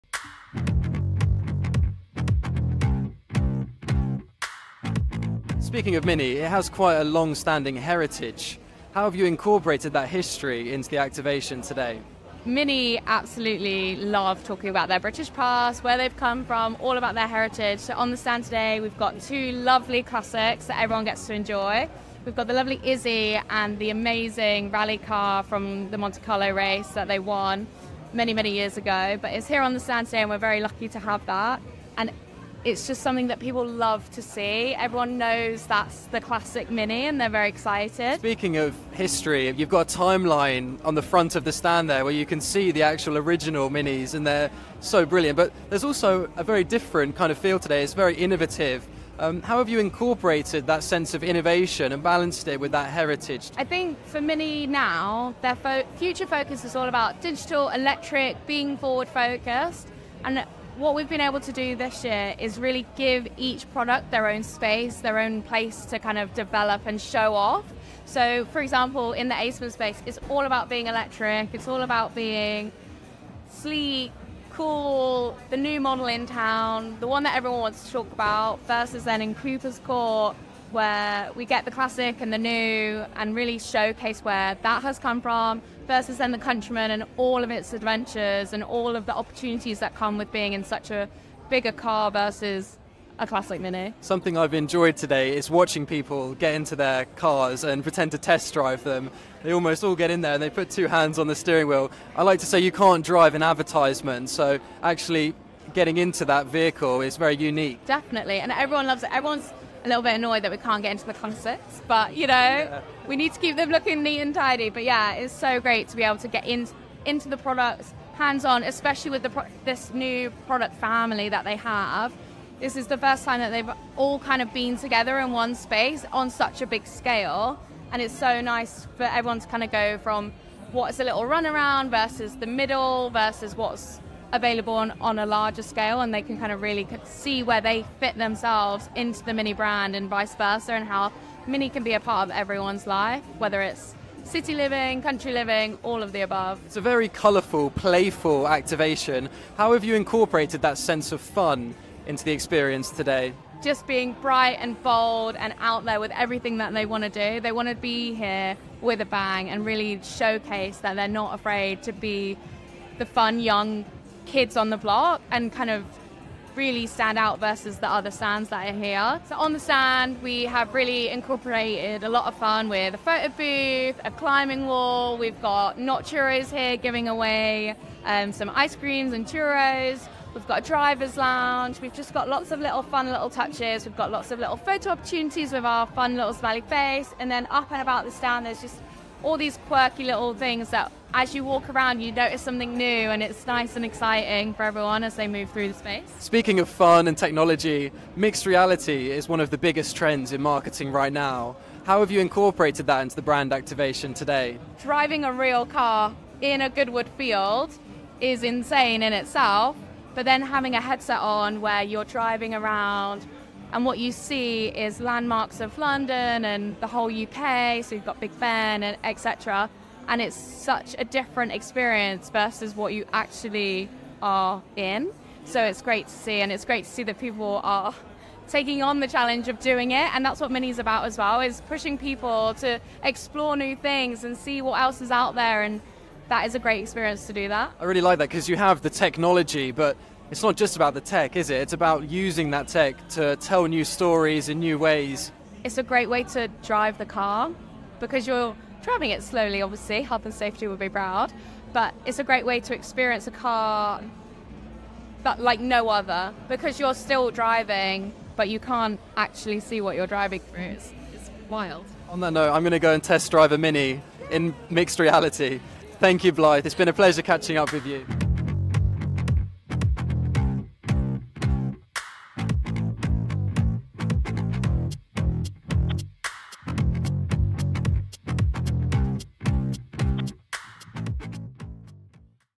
MINI Wows at Goodwood 2024 with Playful Mixed Reality
Explore the vibrant world of MINI at the Goodwood Festival of Speed with IPM's exclusive interview featuring Blythe Grieve, the Project Director. Discover how MINI's stand, bursting with colour and playful architecture, celebrates the brand's rich British heritage while embracing modern advancements. Learn about the unique features like the mixed reality test drive experience, basketball hoops, and tasty treats that redefine visitor engagement.